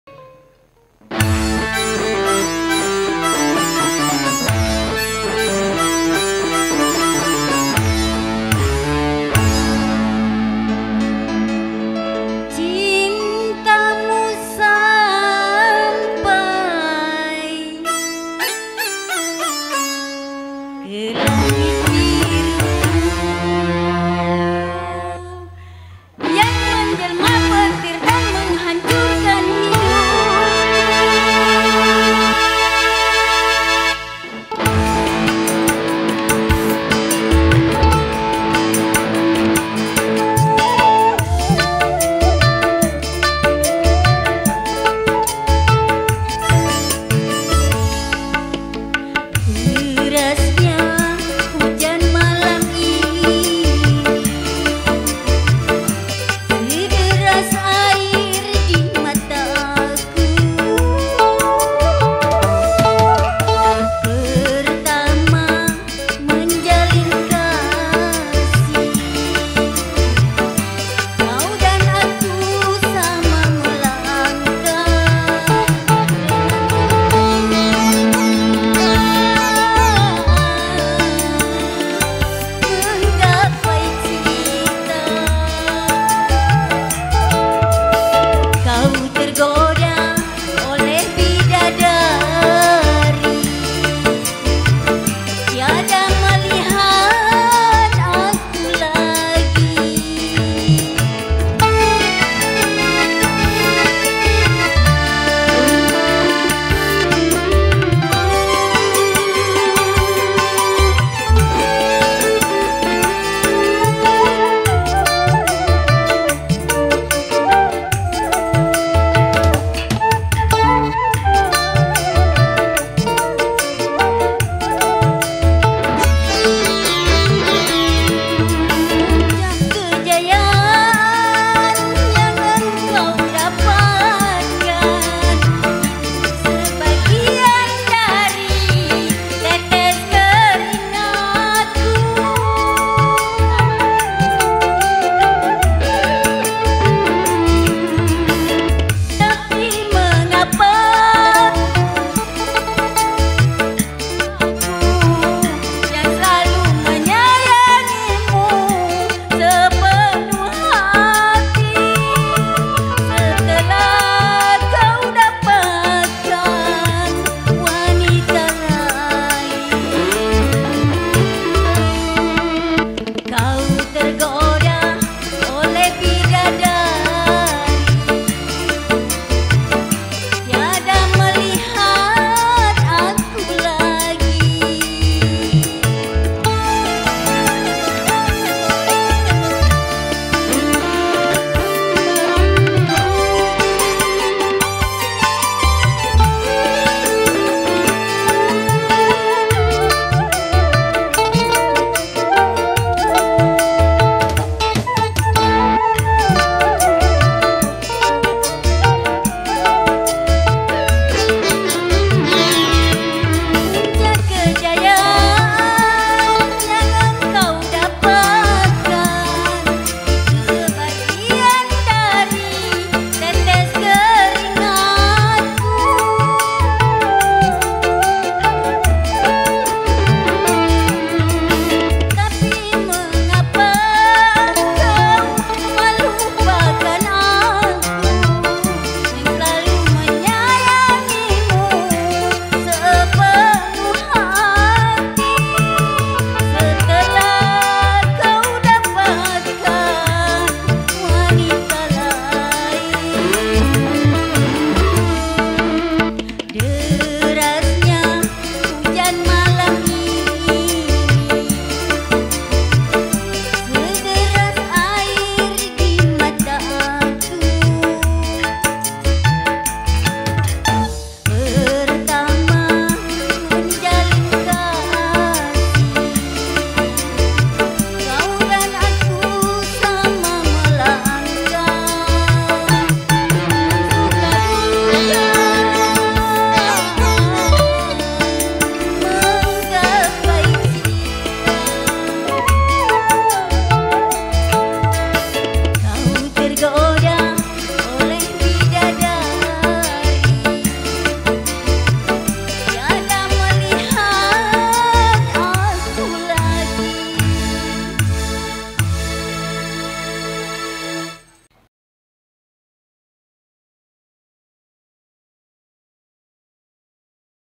COLIN'S MUSIK SHOW DESA SUGI RAYA
Acara Resepsi Pernikahan Emi Fitria & Agus Putra Saba Hari Rabu  23 Agustus 2023 Bertempat di Desa Sugi Raya  Kec. Babat Toman - MUBA Dihibur Oleh COLIN'S Music dari Kel. Mangun Jaya

Di Dokumentasikan Oleh Mutiara Indah Studio Desa Kasmaran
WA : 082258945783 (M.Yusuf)